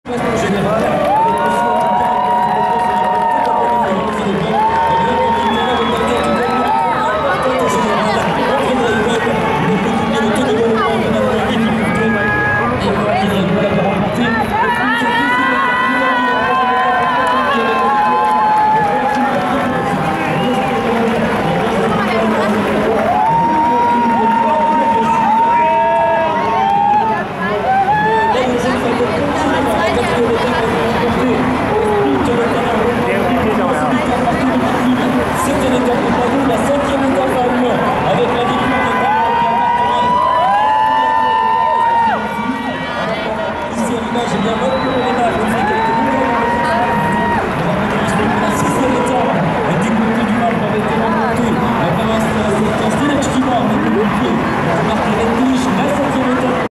Tour de France - Arrivée aux Champs Élysées Arc de Triomphe - Paris - 2015 #3
Un autre passage des coureurs à l'arc de Triomphe